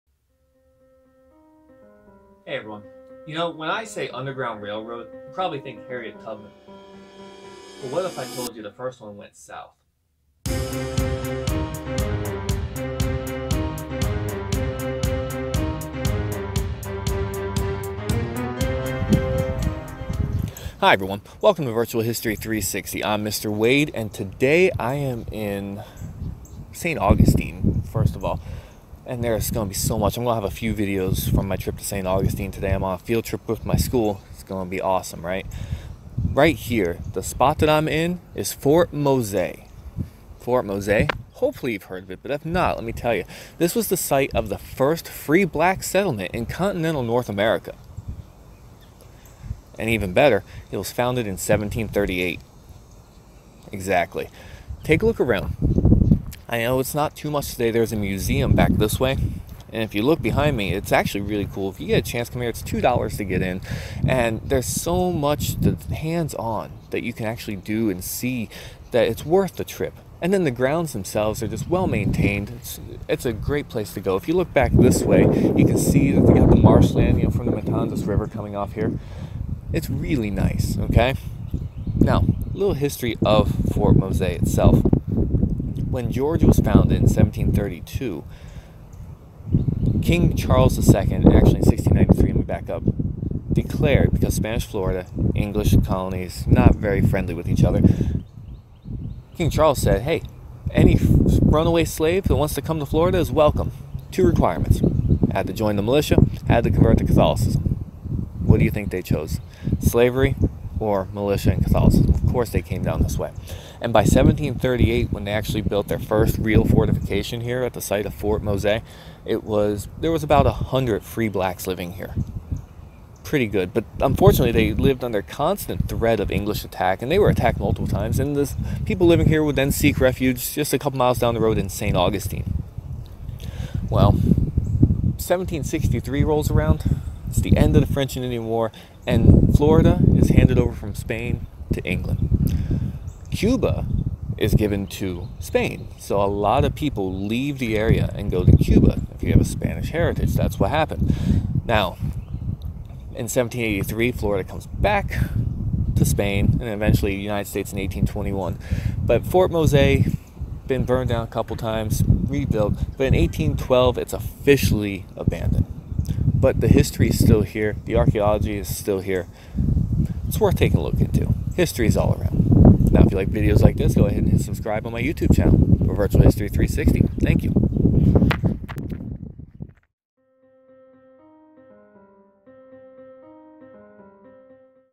First Underground Railroad - Fort Mose
Which direction did the first Underground Railroad go?  Did you say South?  I hope so because the first free black settlement in North America was in Florida.  Check out this 360 video talking about Fort Mose.